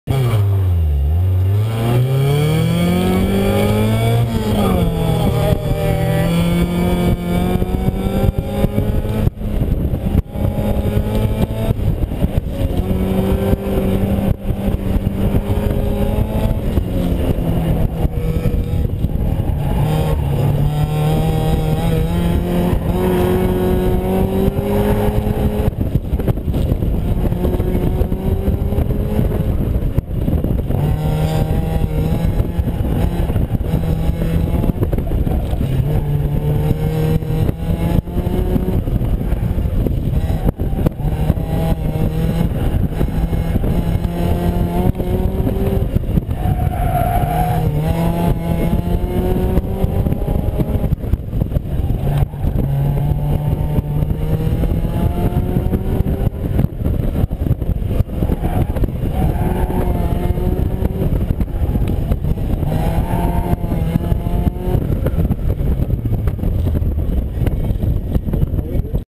NE Divs - Ben Wagstaff - Day 1 - Run 4
Ben Wagstaff drives his STS Miata on Day 1 of the SCCA Solo North East Divisional Championship on 7/30/2011 at Seneca Army Depot in Romulus, NY.